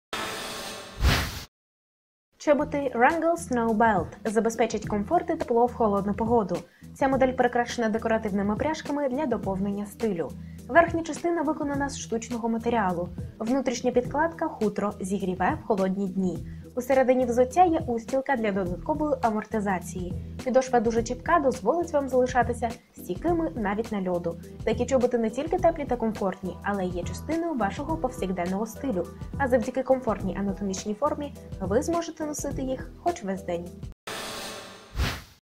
Сапоги Wrangler SNOW BELT WL182670-062
Чоботи Wrangler SNOW BELT

Чоботи Wrangler SNOW BELT забезпечать комфорт і тепло в холодну погоду. Ця модель прикрашена декоративними пряжками для доповнення стилю. Верхня частина виконана з штучного матеріалу. Внутрішня підкладка - хутро, зігріваючий в холодні дні. Усередині взуття є устілка для додаткової амортизації. Підошва дуже чіпка дозволить вам залишатися стійкими навіть на льоду. Такі чоботи не тільки теплі та комфортні, але й є частиною вашого повсякденного стилю. А завдяки комфортній анатомічній формі ви зможете їх носити хоч весь день.

Матеріал: еко-шкіра
Колір: чорний